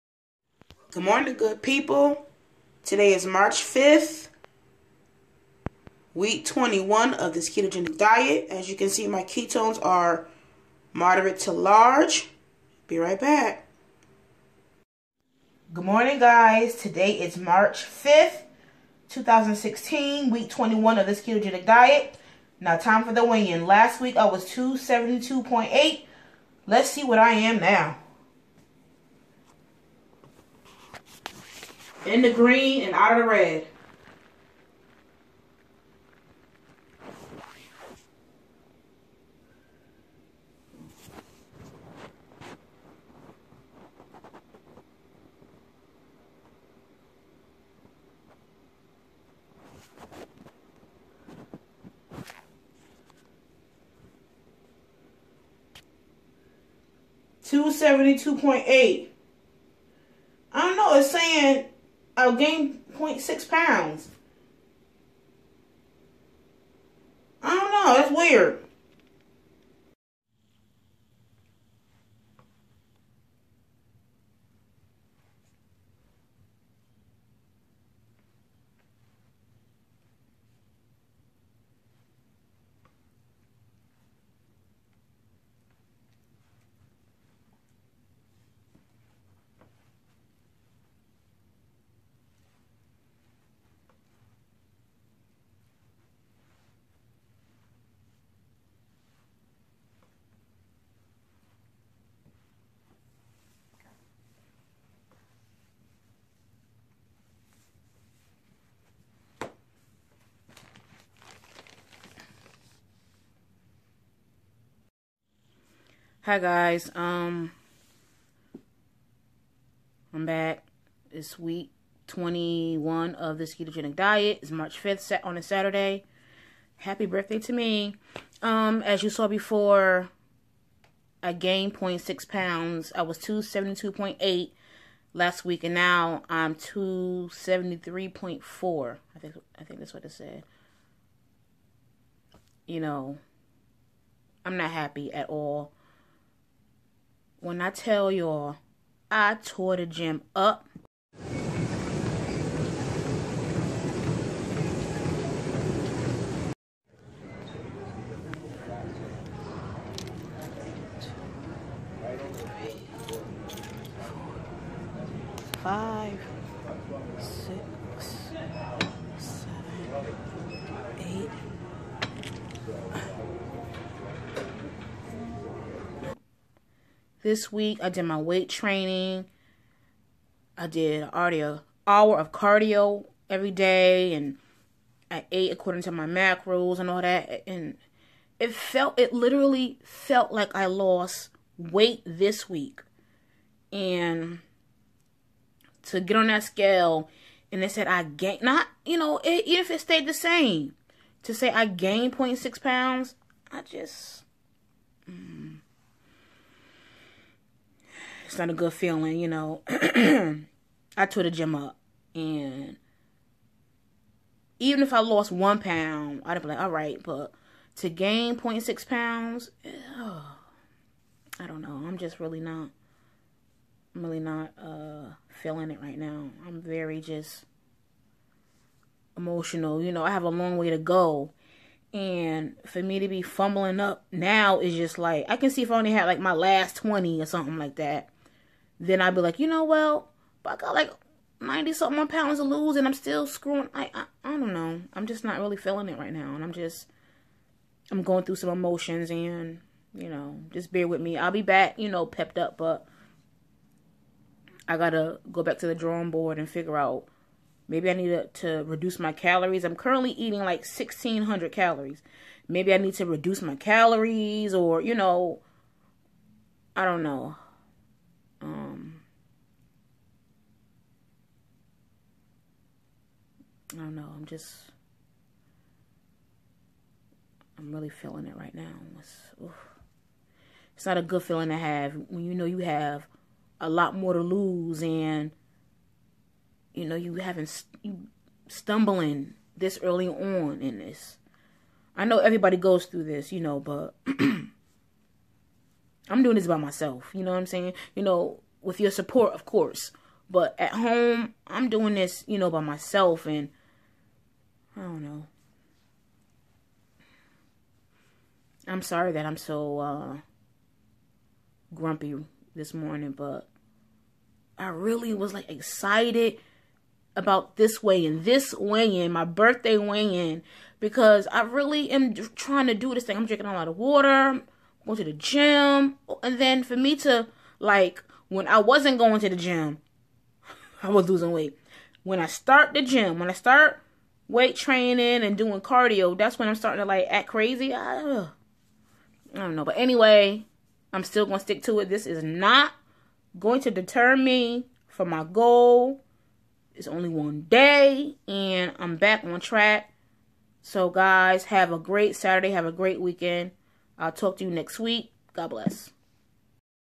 Ketogenic Diet/ Weight loss Journey :Week 21 HBD to MEEEEE !!!!
DISSAPOINTED WITH RESULTS THIS WEEK BUT. ILL BE ALRIGHT